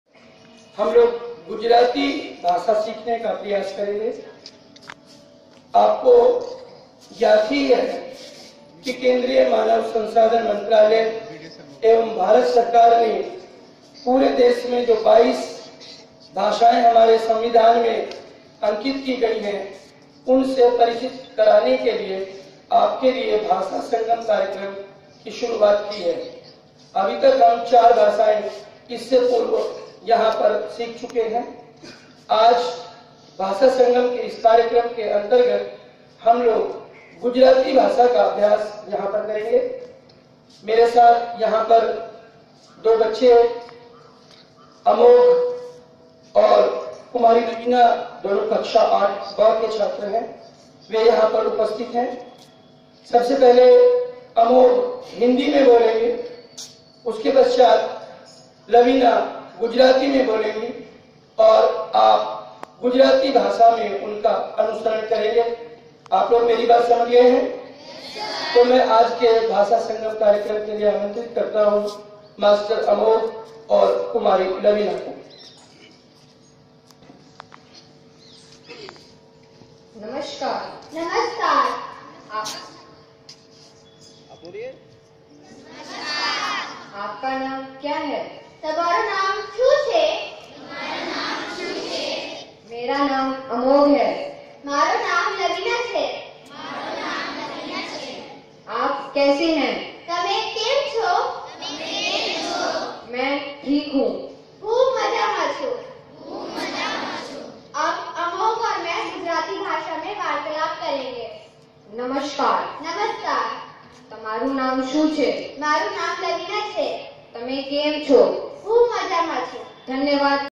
Bhasha Sangam Gujrati language Kendriya Vidyalaya No.02 Roorkee
Under Bhasha Sangam Gujrati language presented by the Students